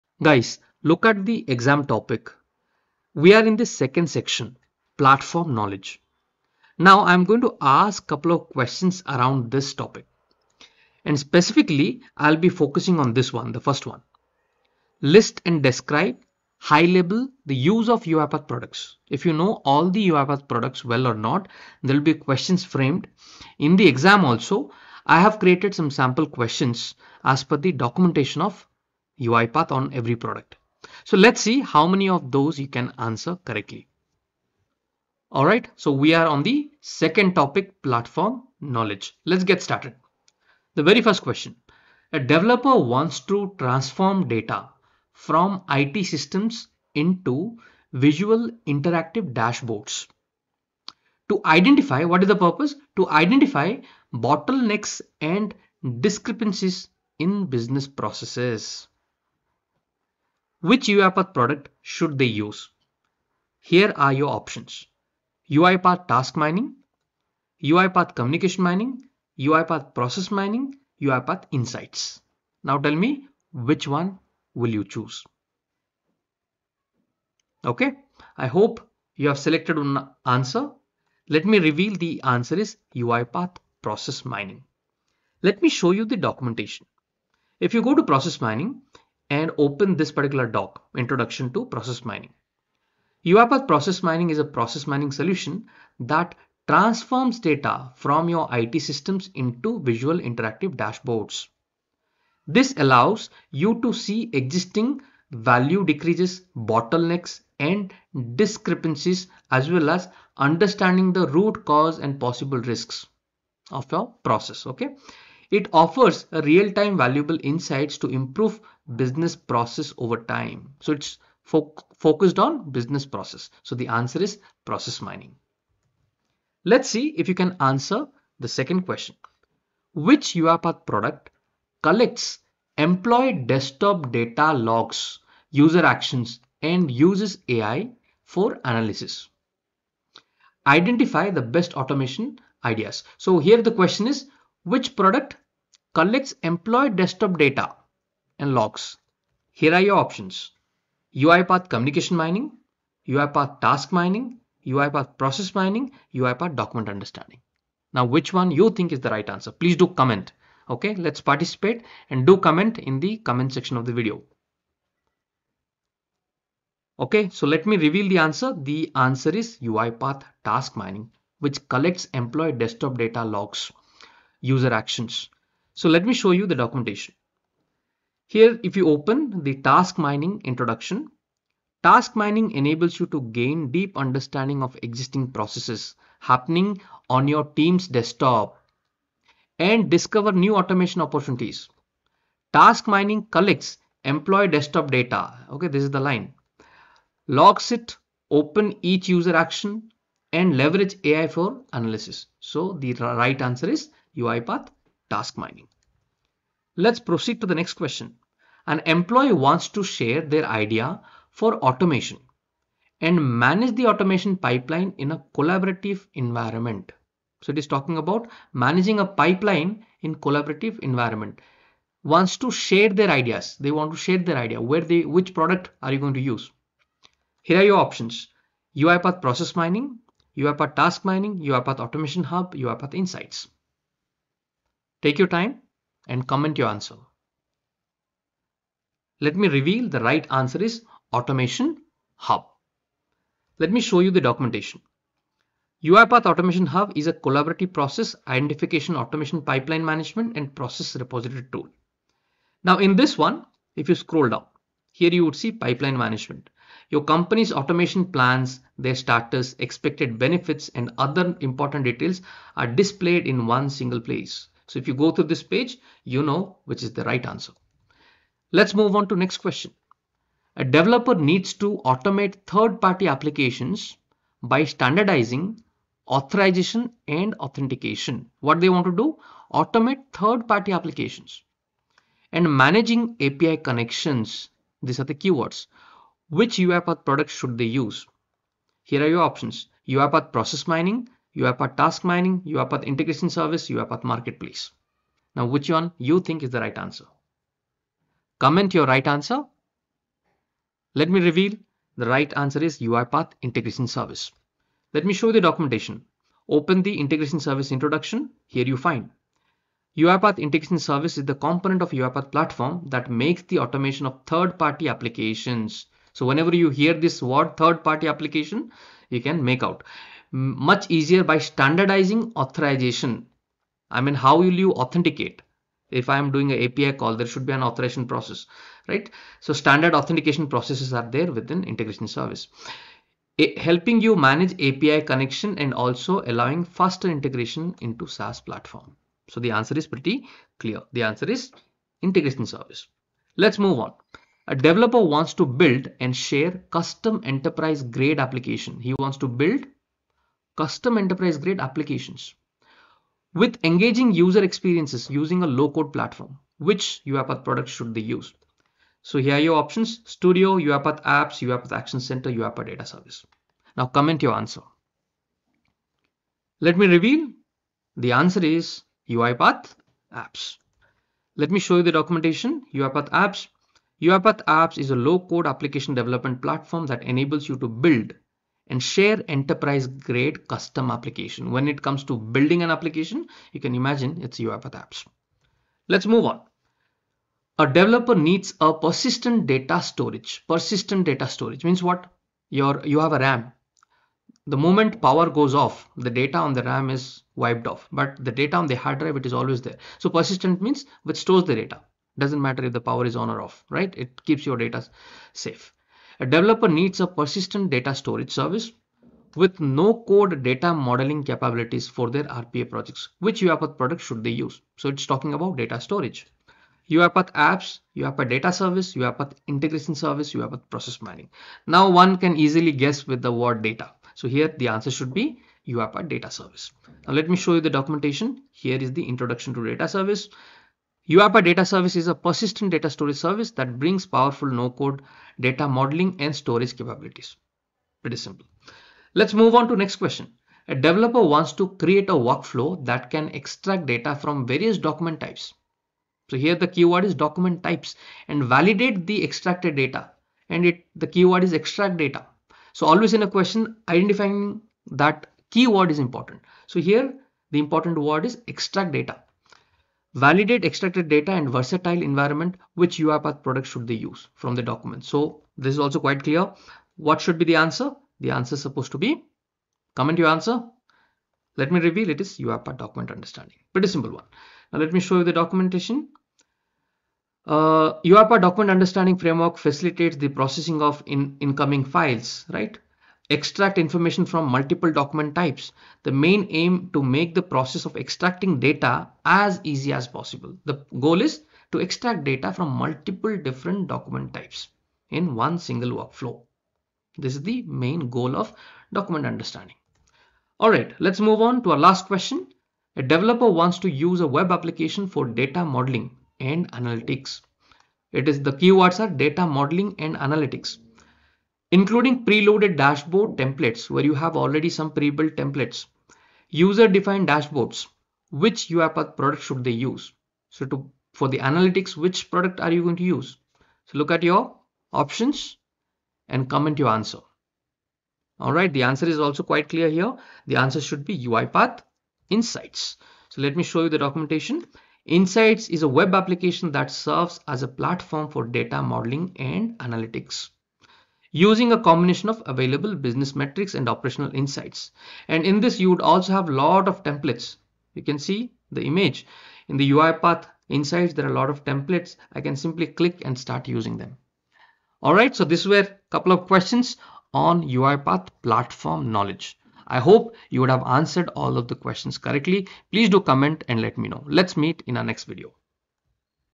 UiPath Platform Products | Q&A on Products of UiPath Platform
In this video, you will test your knowledge on UiPath platform products. We'll cover questions on Process Mining, Task Mining, Automation Hub, Insights, UiPath Apps, Integration Service, and more. Challenge yourself and see how well you understand UiPath’s suite of tools and their applications.